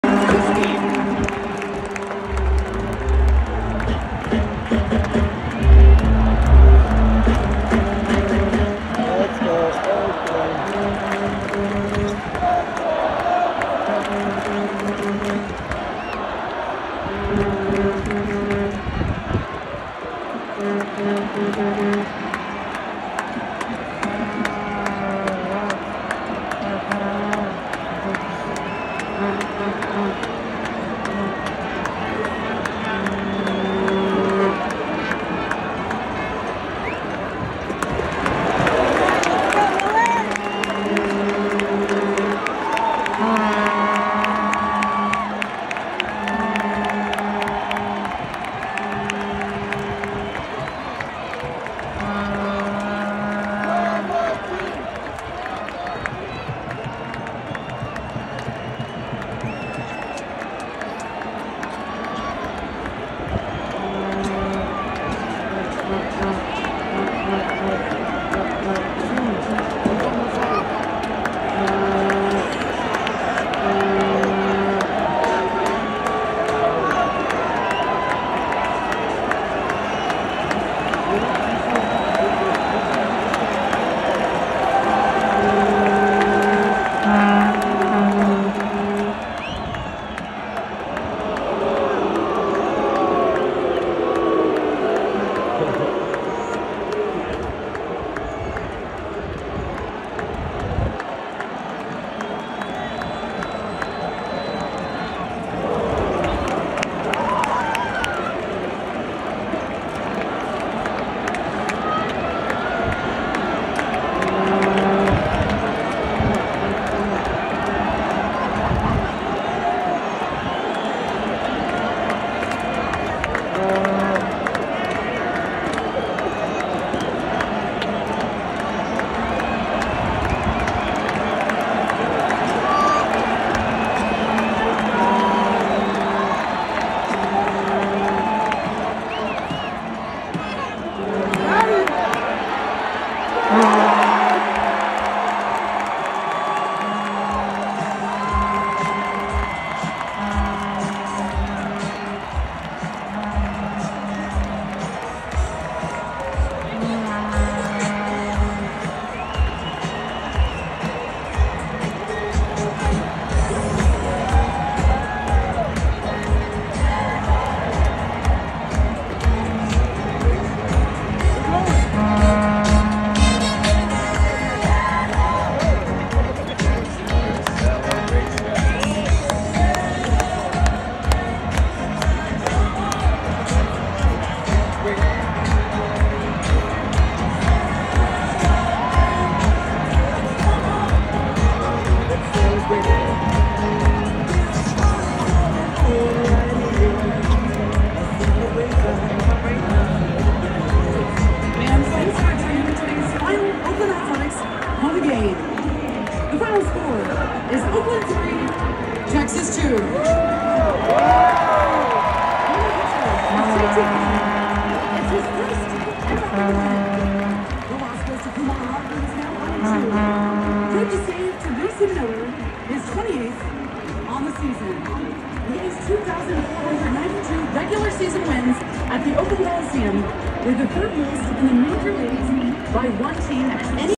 Final at-bat in Oakland Athletics history - 9/26/24
The final major league at bat at the Oakland Coliseum. Texas Rangers’ Travis Jankowski grounds out to Oakland A’s shortstop Jacob Wilson with the throw to first baseman Tyler Soderstrom. Mason Miller got the save to end the final Oakland A’s game at the Coliseum.